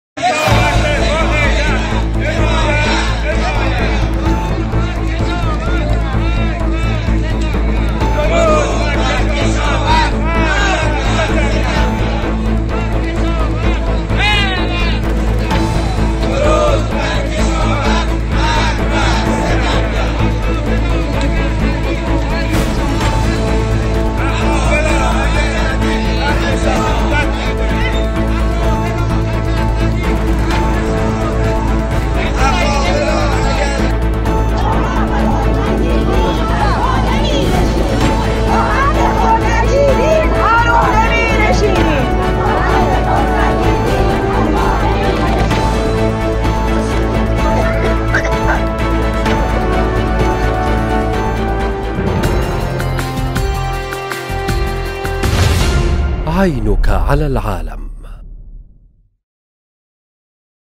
احتجاجات أصفهان تندد بـ "عار التجاهل الحكومي"
العين الإخبارية | احتجاجات أصفهان تندد بـ "عار التجاهل الحكومي"
واصل محتجون إيرانيون مظاهراتهم لليوم الثاني على التوالي بمحافظة أصفهان الواقعة وسط البلاد، بسبب سوء الوضع المعيشي، فضلا عن تزايد معدلات شح المياه العذبة بالإقليم، منددين بما عدوه "عار التجاهل الحكومي"